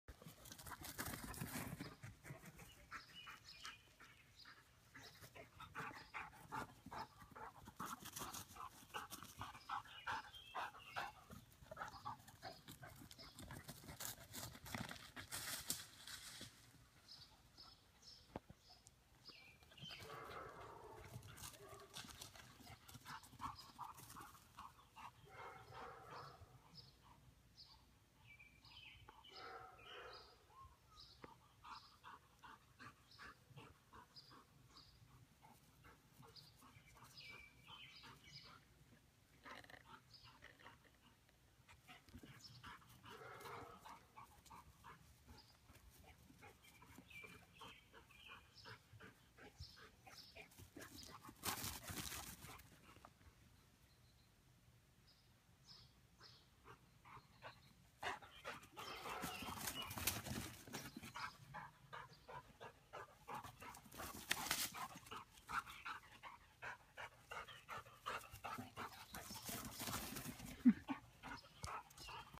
Deer Chase
This is my dog (Brutus) chasing (and being chased by) a deer. This went on for nearly 45 minutes.